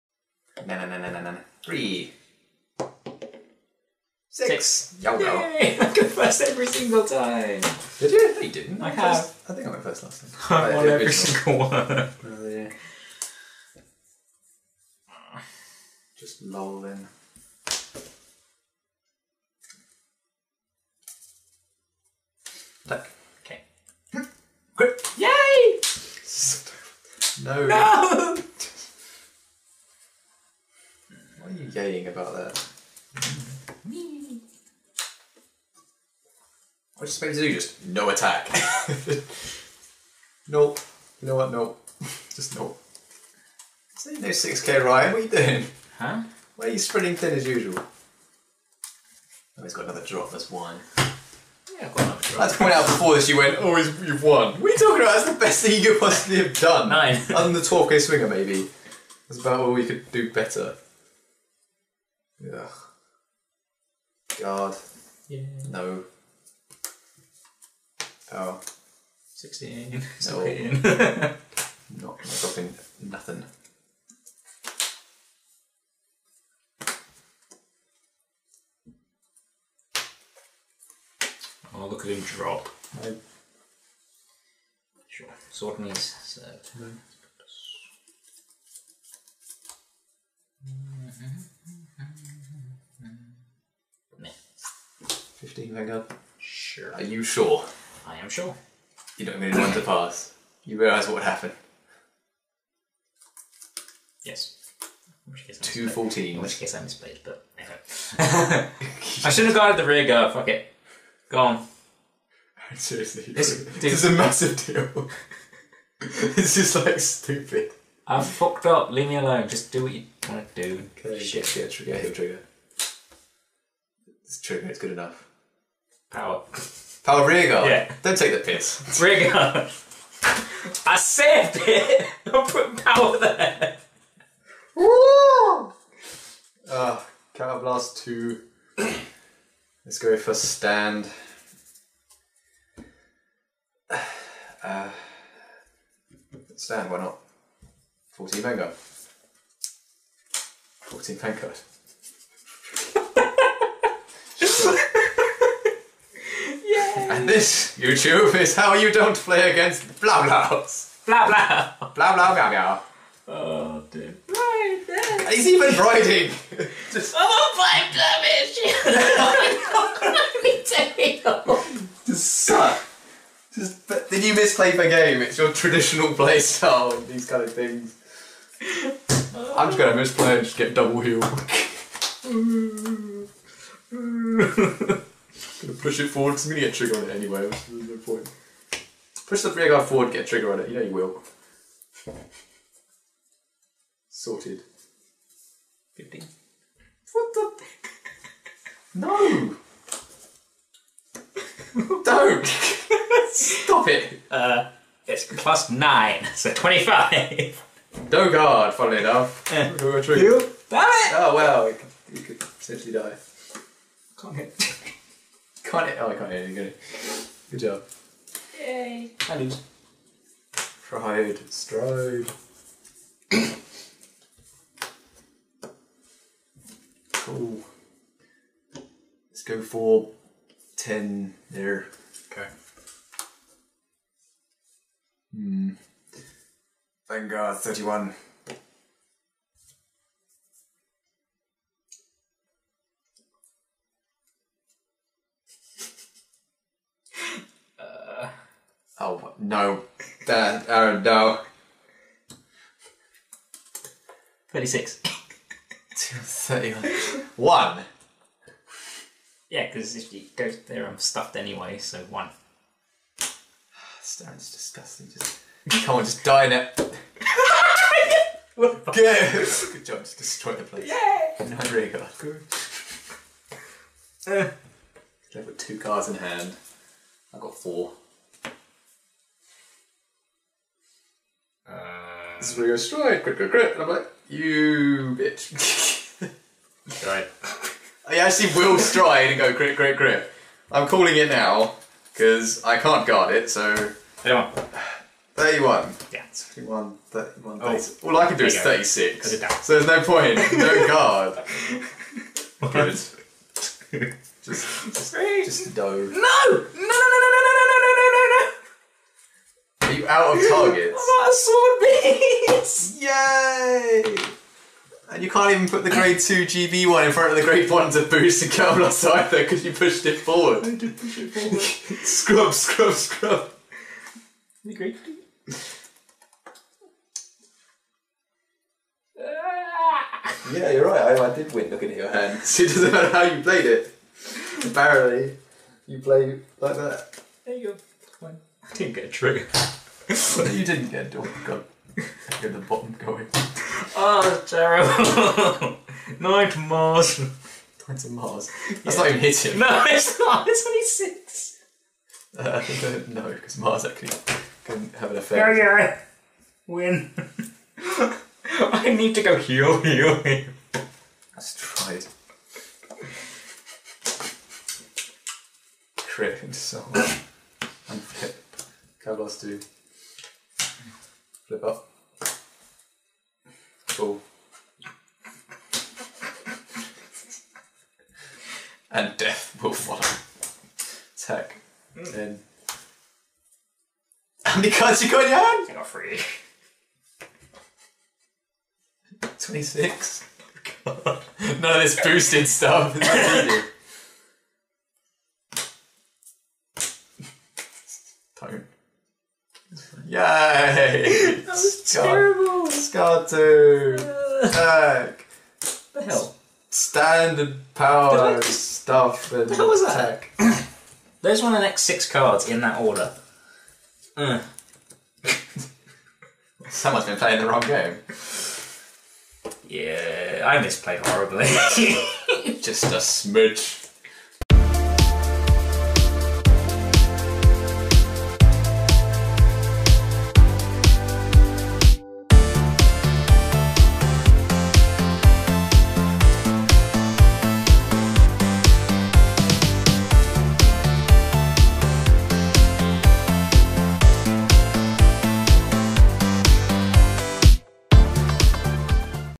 Cardfight!! Vanguard - Nova Grappler (Blau) Vs Royal Paladin (Sanctuary Guard) - Game 3
And we're back!
Been a while, but now we are finally ready to upload some more videos!

This week we just have a little fun with some of our older decks that haven't really been on the channel.
We will hopefully be making more decks, ready for matches and breakdown soon.

Until then, Enjoy!!

Game 3

Mike - Nova Grappler - Blau
Aaron - Royal Paladin - Sanctuary Guard